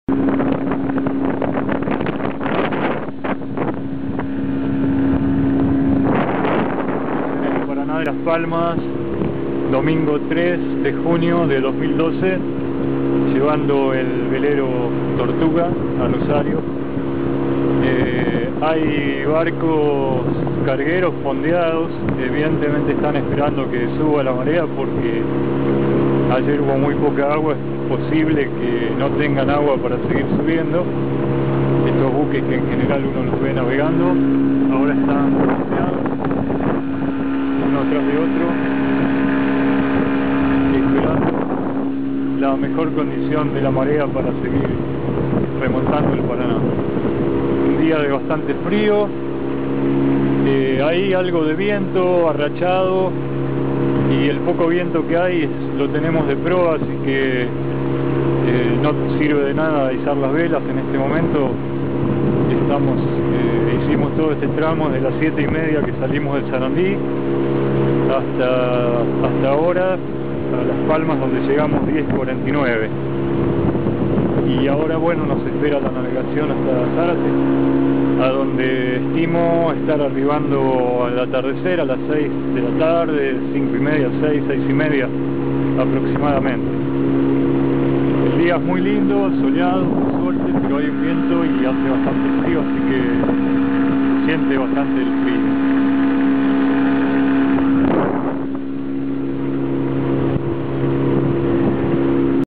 video 2: Paraná de las Palmas cerca de Canal Arias
Domingo 3 de junio de 2012.  Después de navegar por el río Luján hasta el Canal Arias, se sigue por dicho Canal y se llega al Paraná de las Palmas por donde se sigue navegando aguas arriba en busca de Zárate en este caso.